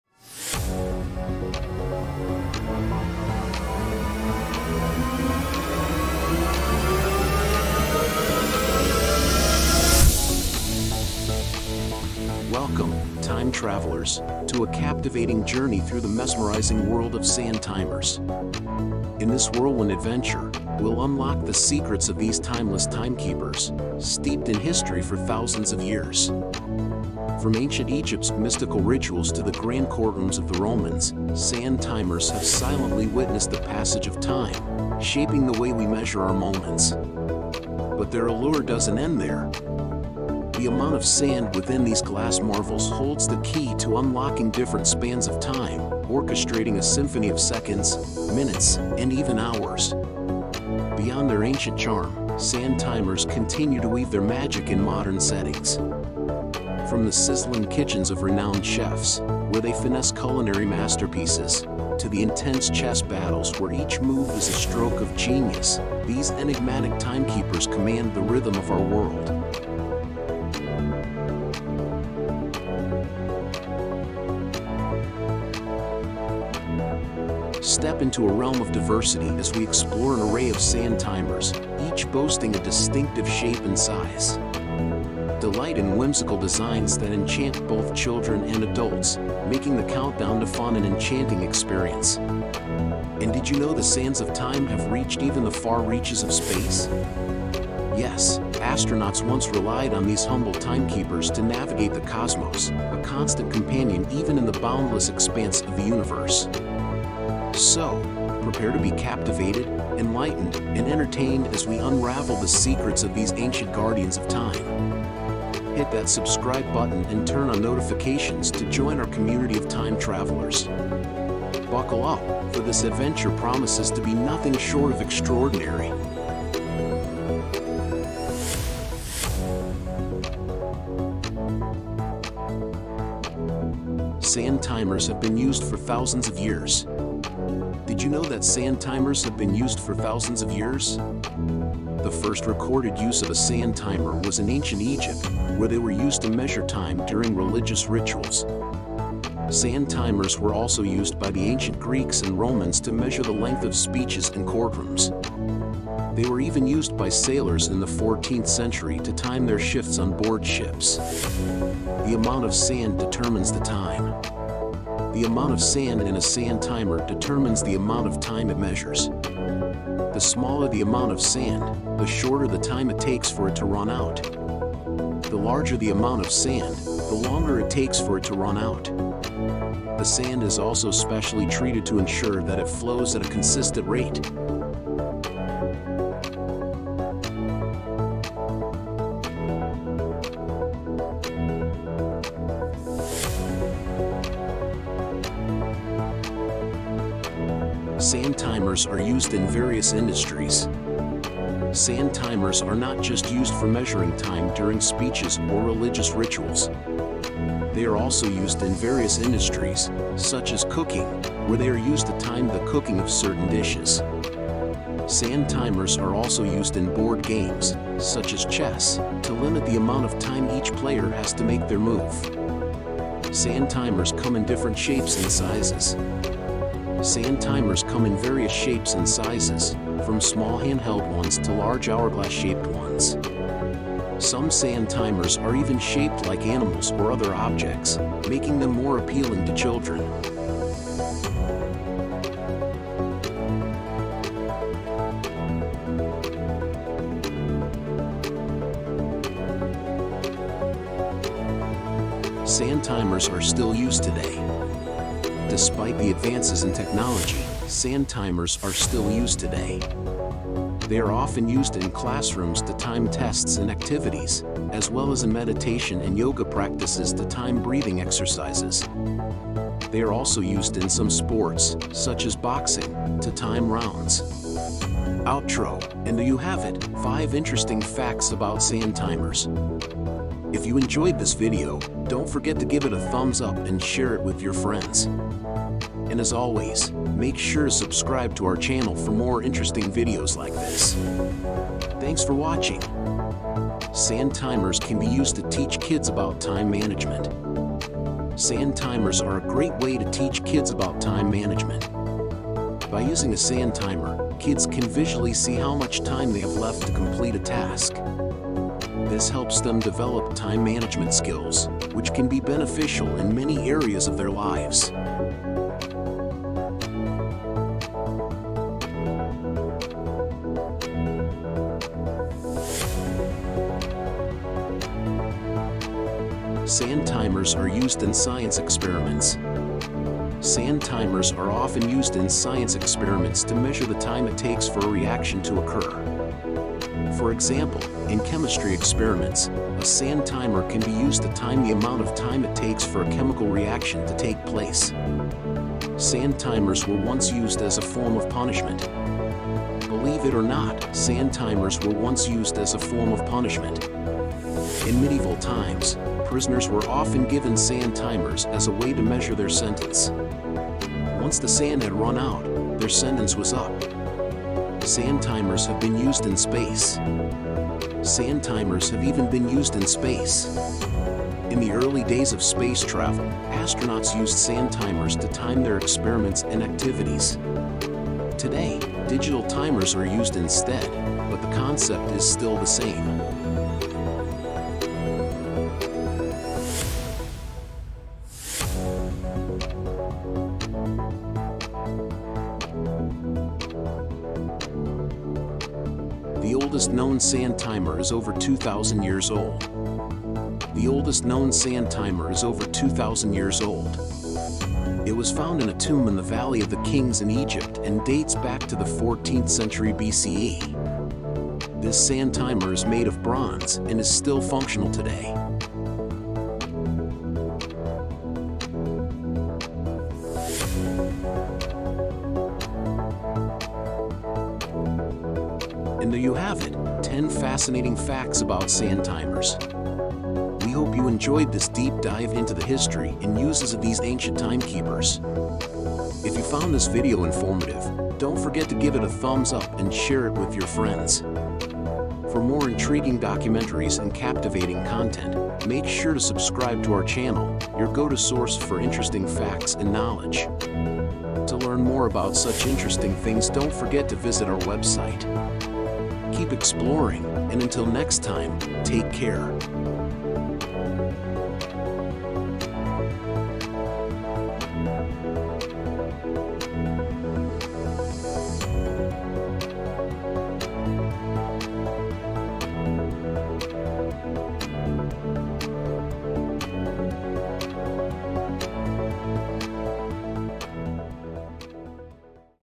Historical Facts About Sand Timers | @factober
Welcome, time travelers, to a captivating journey through the mesmerizing world of sand timers! In this whirlwind adventure, we'll unlock the secrets of these timeless timekeepers, steeped in history for thousands of years.

From ancient Egypt's mystical rituals to the grand courtrooms of the Romans, sand timers have silently witnessed the passage of time, shaping the way we measure our moments. But their allure doesn't end there! The amount of sand within these glass marvels holds the key to unlocking different spans of time, orchestrating a symphony of seconds, minutes, and even hours.

Beyond their ancient charm, sand timers continue to weave their magic in modern settings. From the sizzling kitchens of renowned chefs, where they finesse culinary masterpieces, to the intense chess battles where each move is a stroke of genius – these enigmatic timekeepers command the rhythm of our world.

Step into a realm of diversity as we explore an array of sand timers, each boasting a distinctive shape and size. Delight in whimsical designs that enchant both children and adults, making the countdown to fun an enchanting experience.

And did you know the sands of time have reached even the far reaches of space? Yes, astronauts once relied on these humble timekeepers to navigate the cosmos, a constant companion even in the boundless expanse of the universe.

So, prepare to be captivated, enlightened, and entertained as we unravel the secrets of these ancient guardians of time. Buckle up, for this adventure promises to be nothing short of extraordinary!

Sand Timers Have Been Used for Thousands of Years
Did you know that sand timers have been used for thousands of years? The first recorded use of a sand timer was in ancient Egypt, where they were used to measure time during religious rituals. Sand timers were also used by the ancient Greeks and Romans to measure the length of speeches in courtrooms. They were even used by sailors in the 14th century to time their shifts on board ships.

The amount of Sand Determines the Time
The amount of sand in a sand timer determines the amount of time it measures. The smaller the amount of sand, the shorter the time it takes for it to run out. The larger the amount of sand, the longer it takes for it to run out. The sand is also specially treated to ensure that it flows at a consistent rate.

Sand Timers Are Used in Various Industries
Sand timers are not just used for measuring time during speeches or religious rituals. They are also used in various industries, such as cooking, where they are used to time the cooking of certain dishes. Sand timers are also used in board games, such as chess, to limit the amount of time each player has to make their move.

Sand Timers Come in Different Shapes and Sizes
Sand timers come in various shapes and sizes, from small handheld ones to large hourglass-shaped ones. Some sand timers are even shaped like animals or other objects, making them more appealing to children.

Sand Timers Are Still Used Today
Despite the advances in technology, sand timers are still used today. They are often used in classrooms to time tests and activities, as well as in meditation and yoga practices to time breathing exercises. They are also used in some sports, such as boxing, to time rounds.

Sand Timers Can Be Used to Teach Kids about Time Management
Sand timers are a great way to teach kids about time management. By using a sand timer, kids can visually see how much time they have left to complete a task. This helps them develop time management skills, which can be beneficial in many areas of their lives.